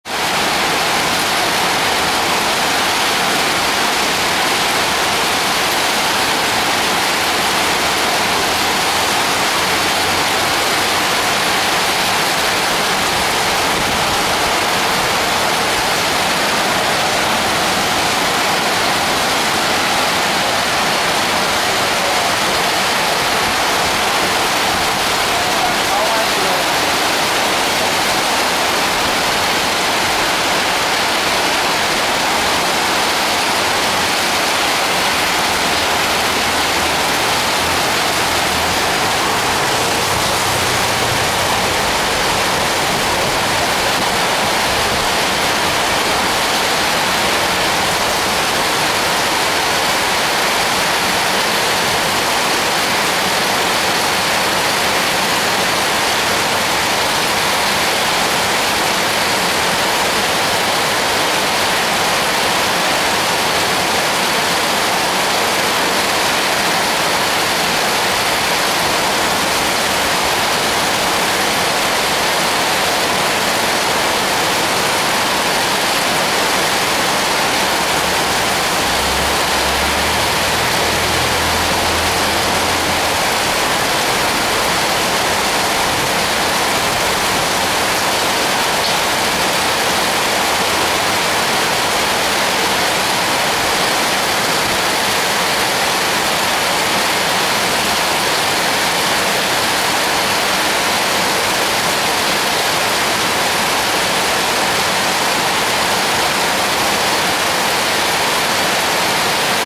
ロイヤルインディアで食べ終わったら土砂降りになった
ロイヤルインディアで食事を食べ終わったら、土砂降りになってしまった。